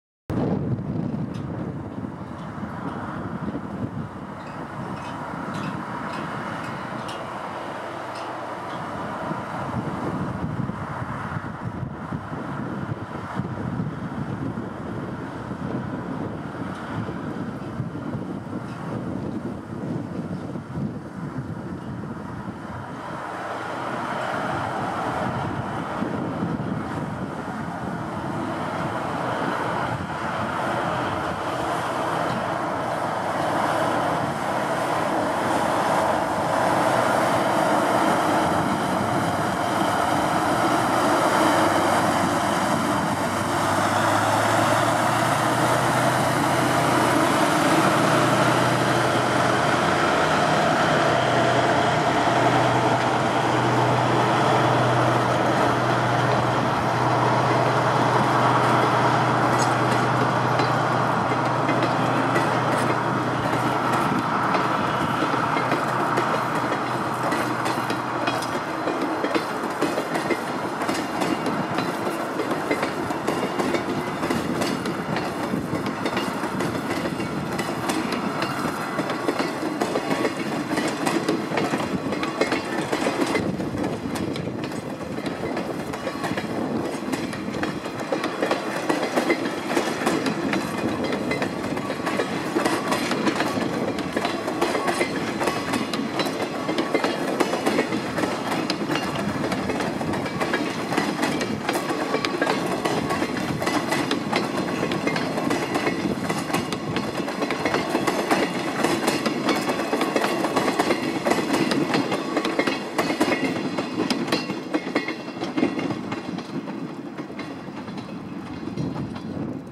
JR貨物 DF200-110貨物列車 RED BEAR 五稜郭駅
2016年撮影
DF200-110レッド ベア牽引　コキ車
函館本線　五稜郭駅ホーム長万部方から